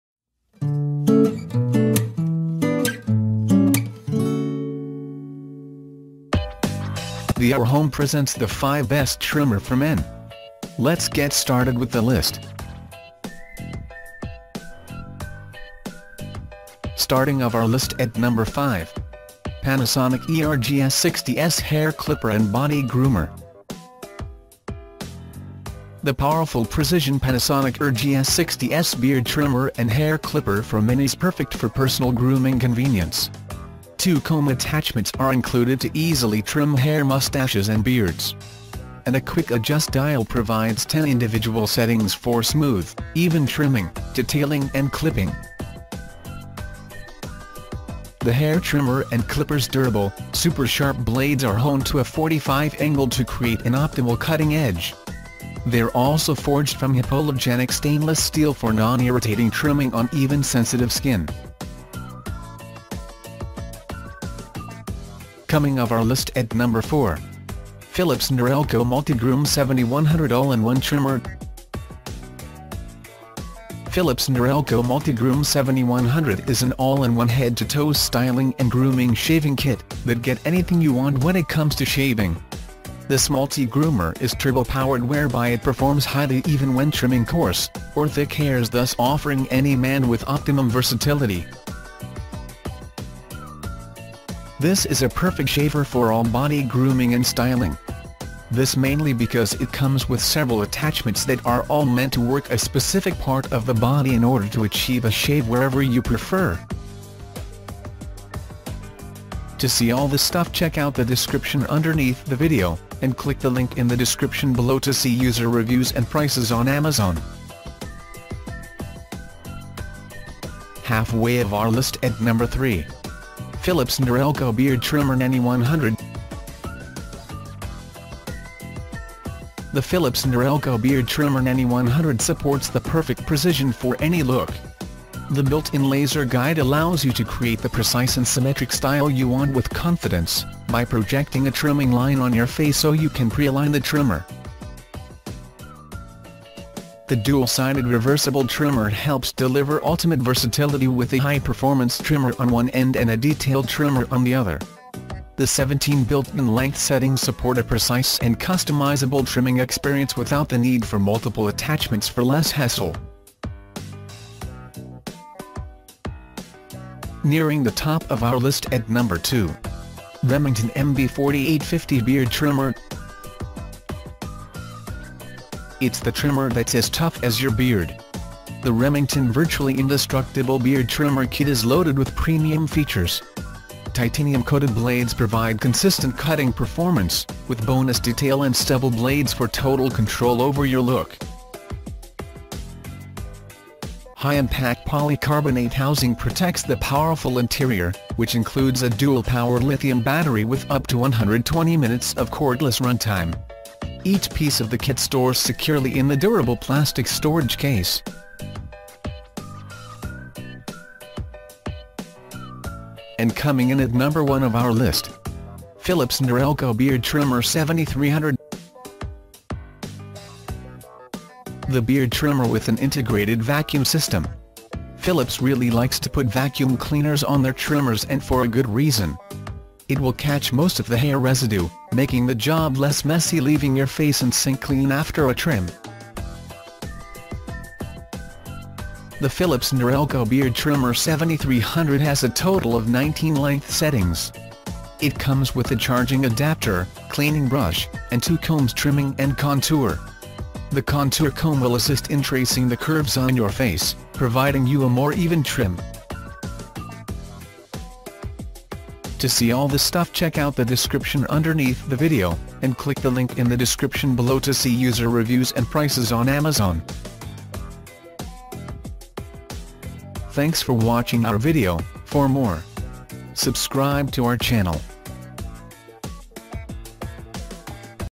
Best Trimmer For Men
Here is the list of 5 Best Trimmer For Men.